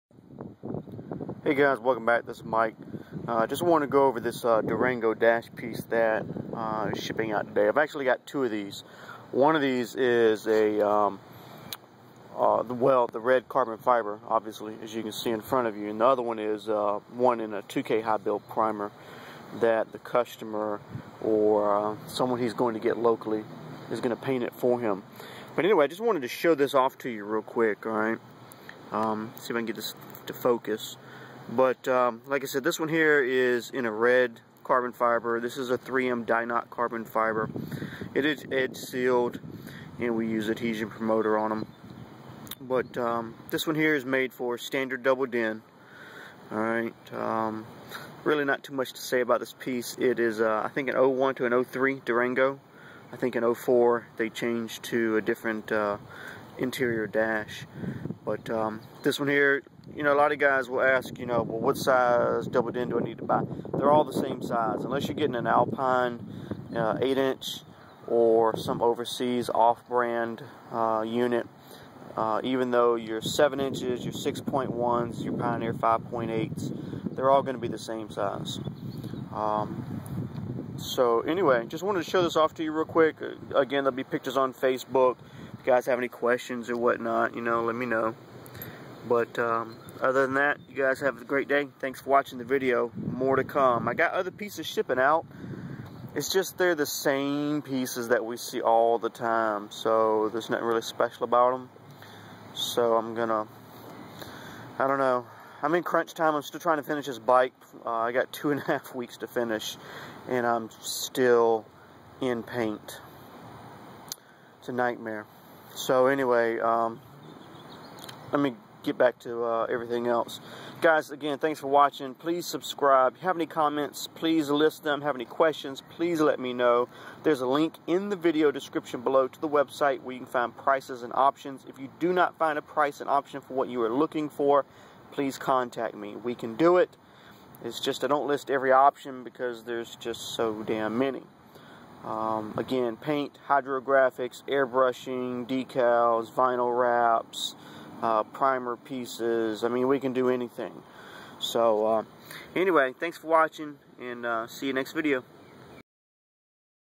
Dodge Durango custom double DIN
This is a custom Dodge Durango double DIN I made for a customer and wrapped in red carbon fiber as requested. It is a perfect fit and looks really killer in person.

If you are looking for a custom piece like this or for something else please let me know or check my site below